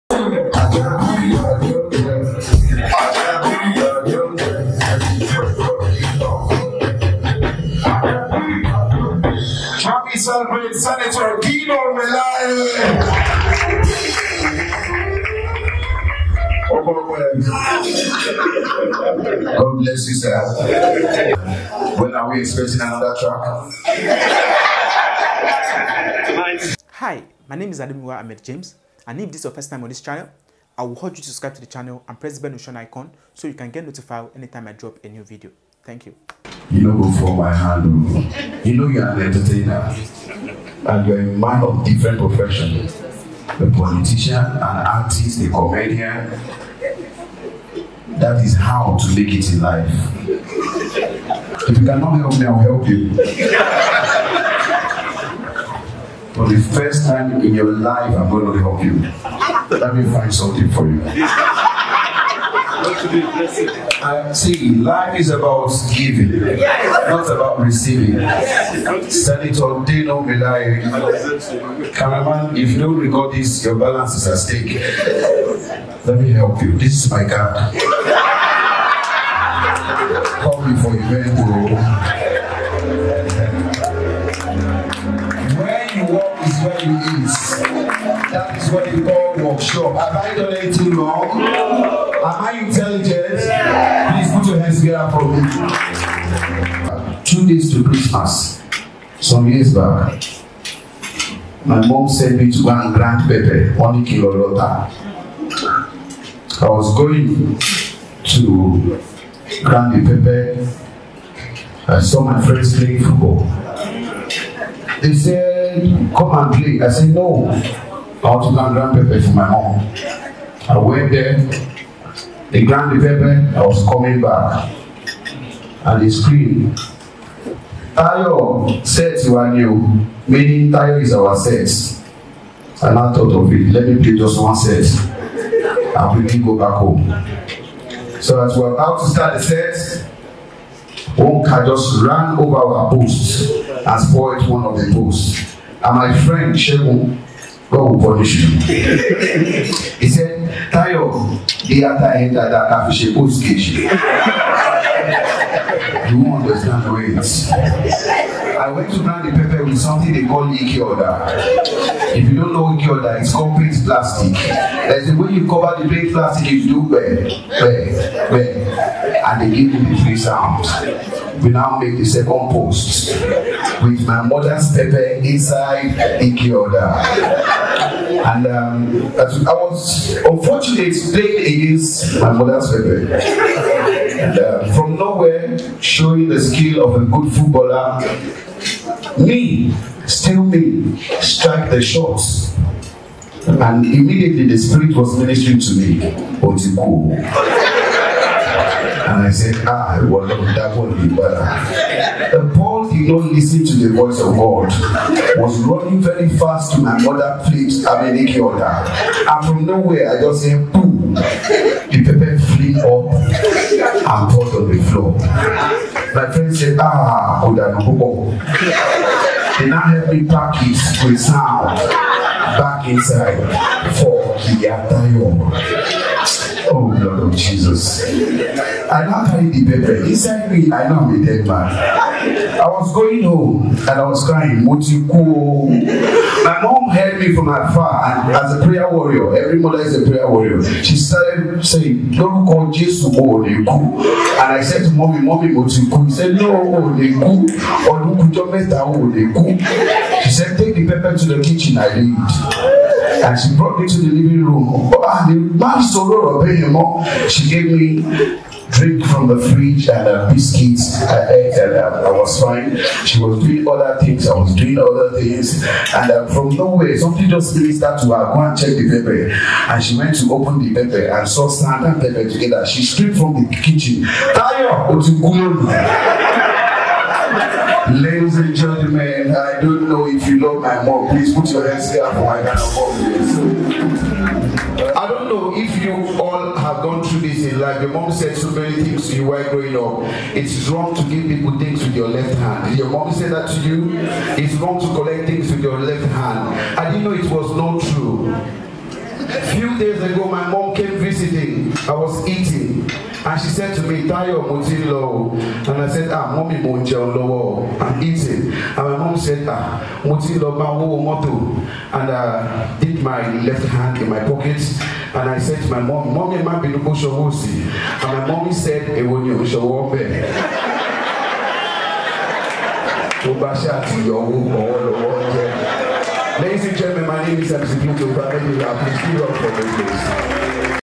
Funny Moments with Senator Dino Melaye || Mc Pluto
Funny Moment with Senator Dino Melaye || Mc Pluto
Senator Dino Melaye got into a funny conversation with an Event Mc